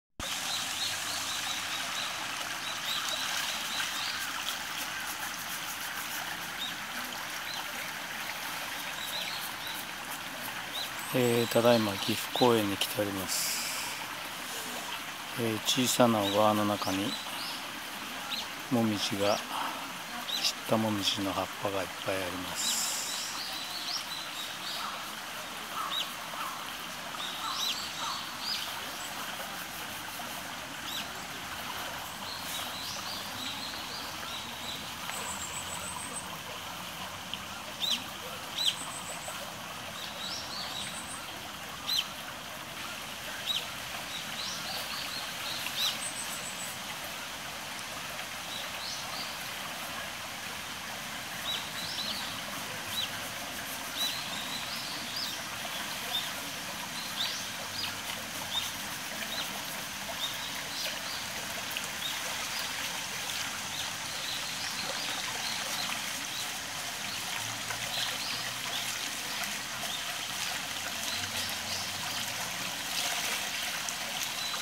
『旅人ゆき』の『岐阜公園内の水辺のモミジ紅葉』Gifu,Japan
2105年11月20日、岐阜市内にある『岐阜公園』内のモミジの紅葉です。
とても綺麗でしたね。